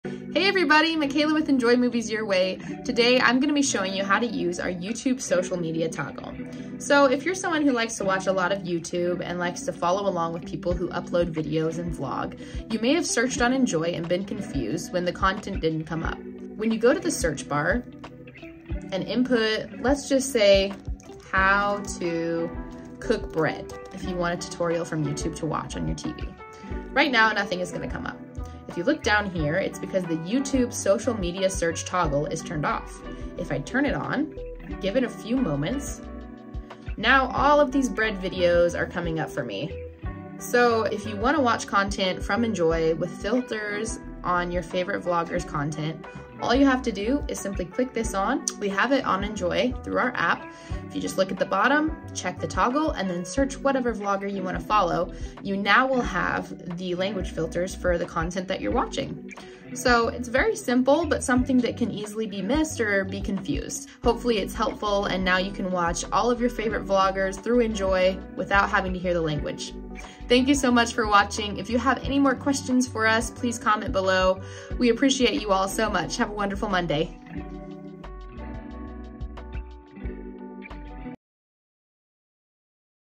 How to Watch all Youtube Social Media Content on the Enjoy App
If you've tried searching your favorite Youtube account through the Enjoy app and were unable to find their videos, here is the simple solution to fix that.